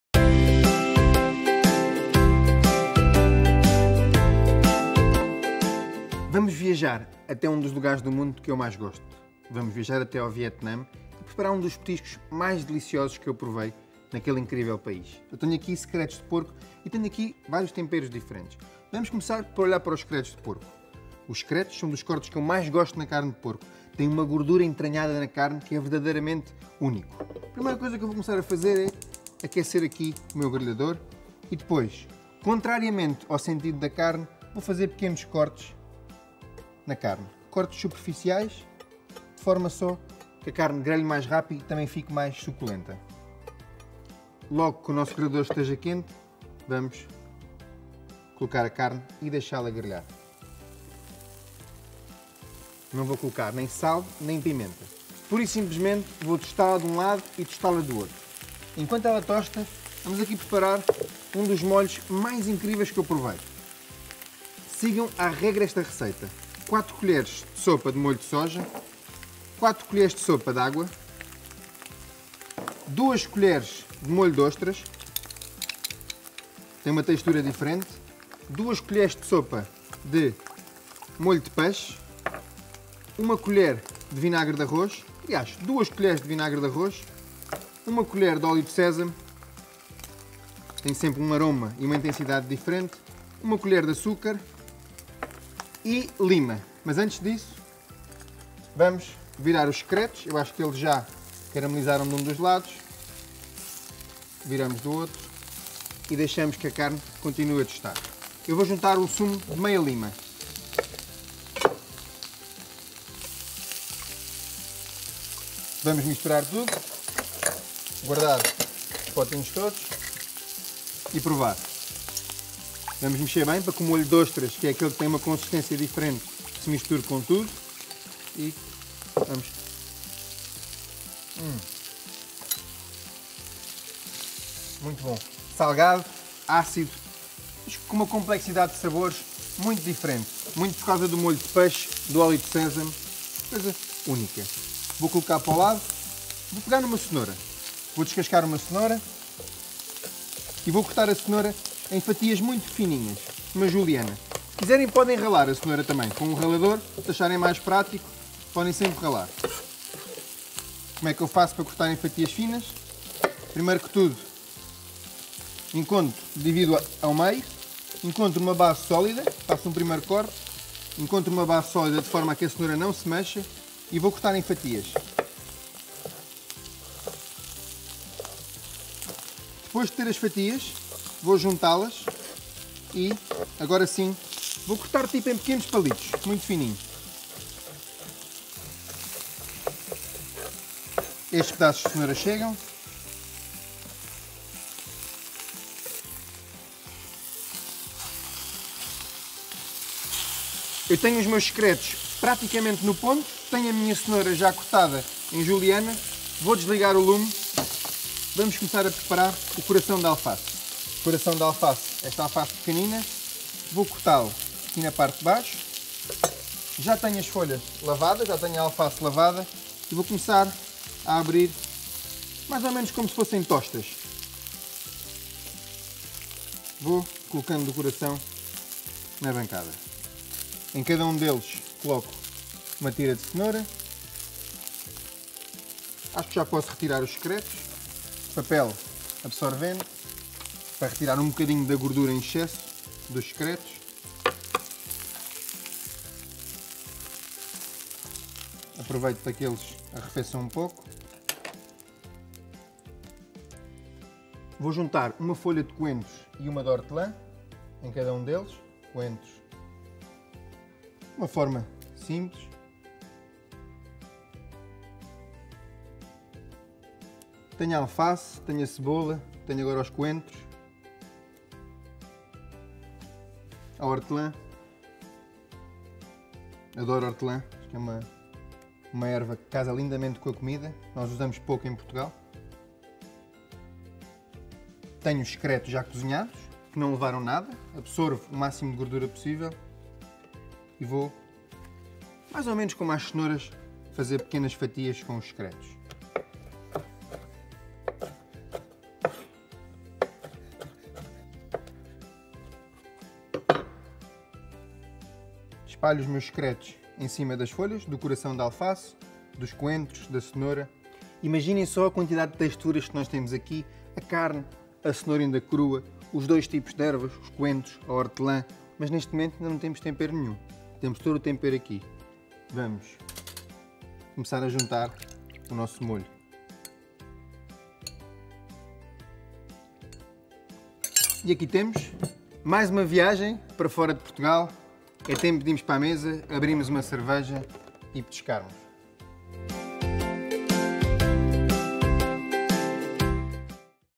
Cozinhas do Mundo: Vietname - Secretos de Porco
Um petisco delicioso preparado com corações de alface recheados com secretos de porco grelhados, cenoura e coentros, temperados com um molho agridoce.